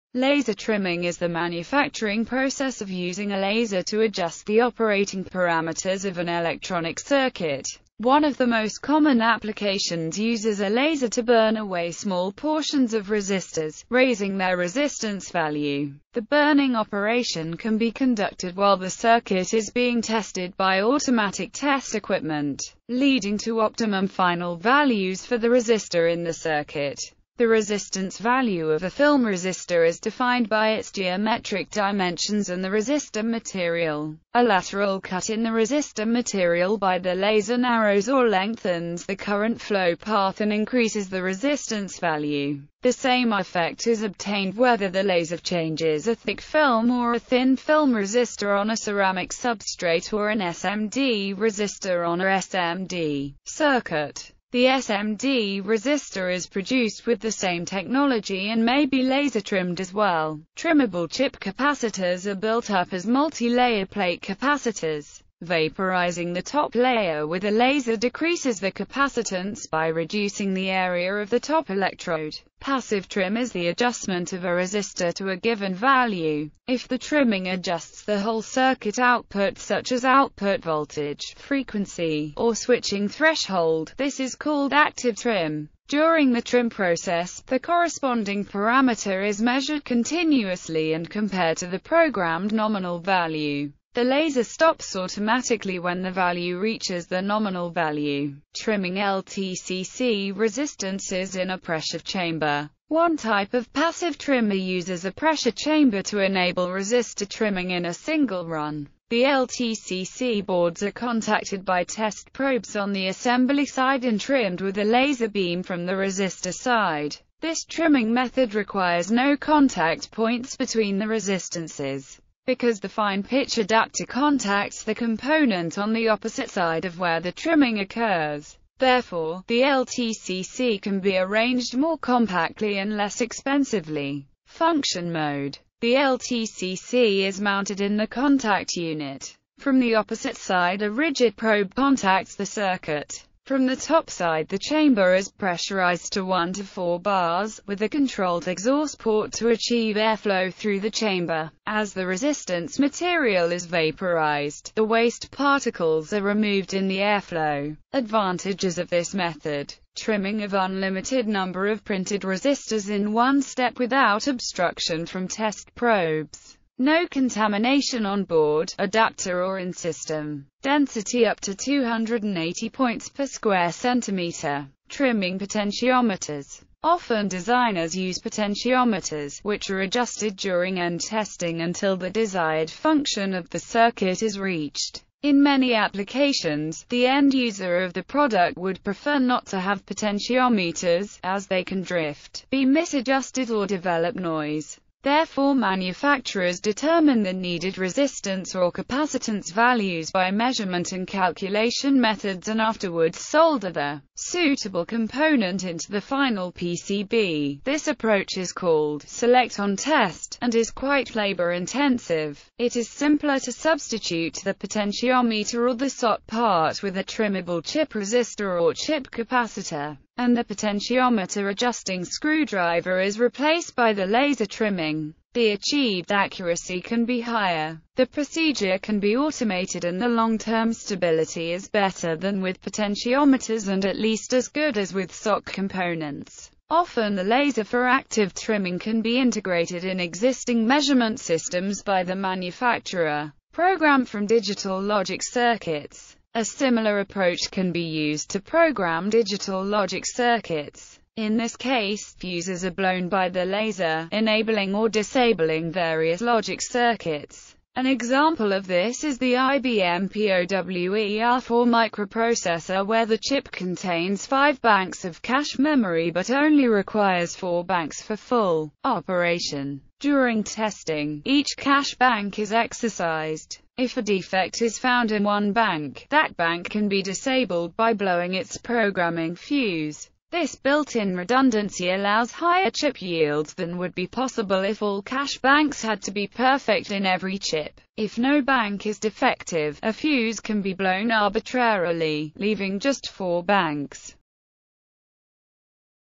Laser trimming
Laser trimming is the manufacturing process of using a laser to adjust the operating parameters of an electronic circuit.One of the most common applications uses a laser to burn away small portions of resistors, raising their resistance value.

Image-Copyright-Info
Author-Info:   Binarysequence

Image-Copyright-Info